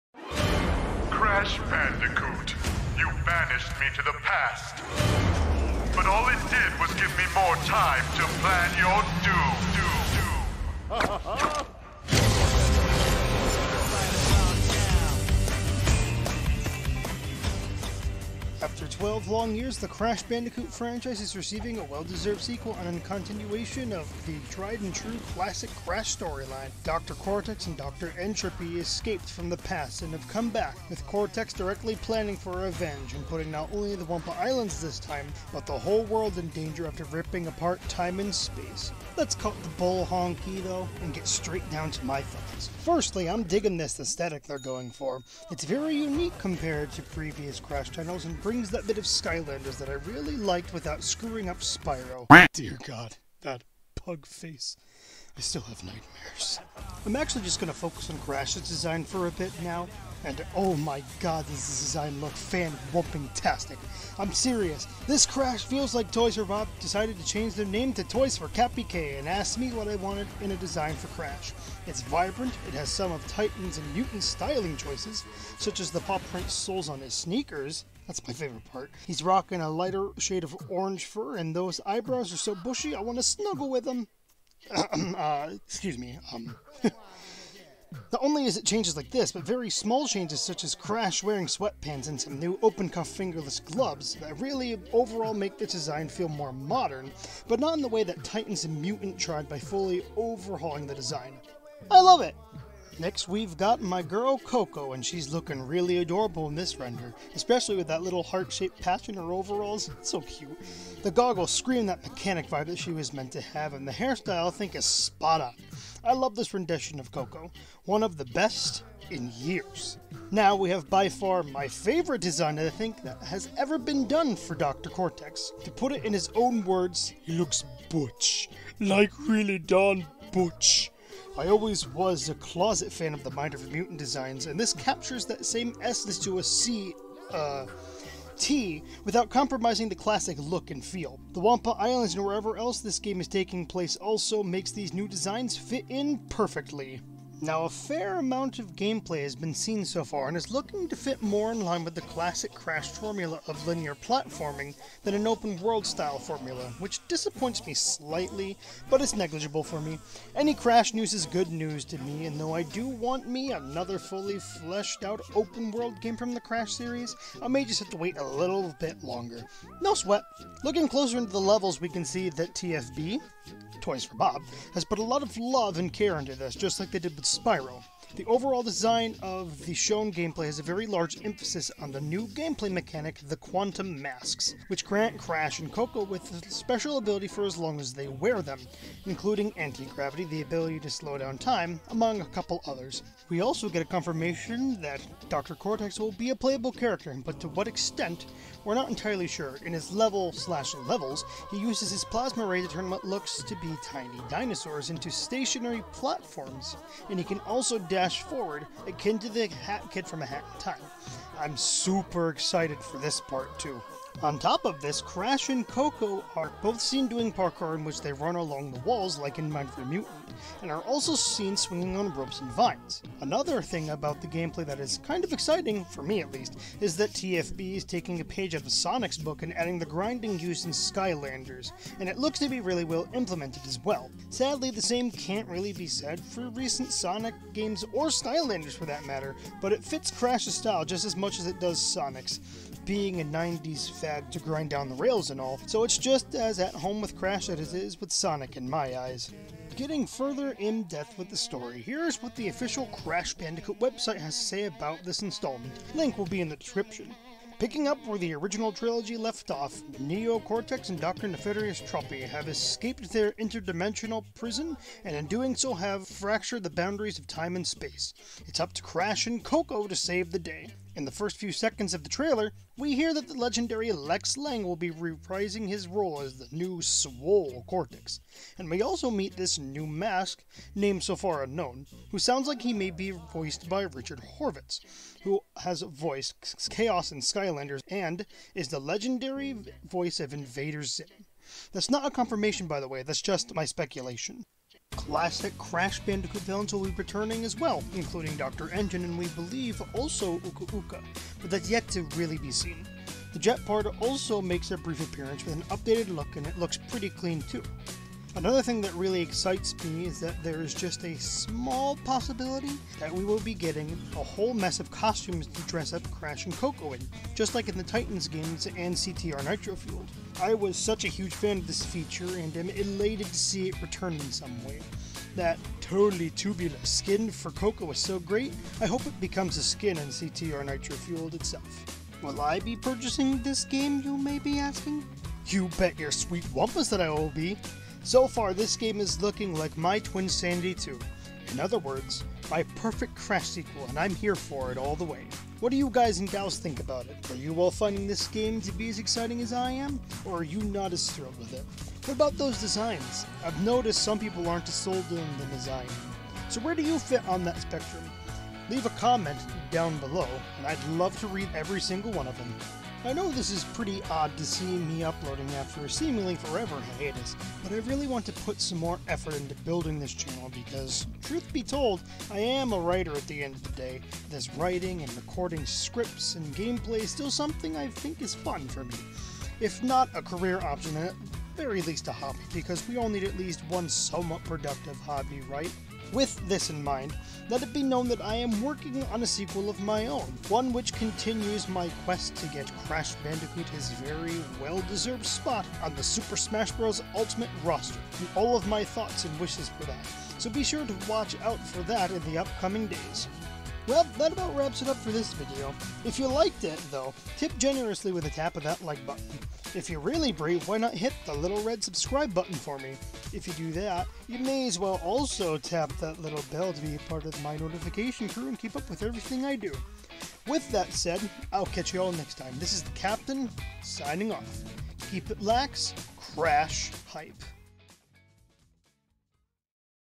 It's About Time? NO KIDDIN'! (My Thoughts on Crash 4 and Some More Info!)
The Crash Bandicoot franchise is getting a sequel. 12 years after the last one... Let's talk about that. This video will be the first of what may become a revitalization of this channel. It has sat dormant too long for my liking and I hope to get back into the groove..!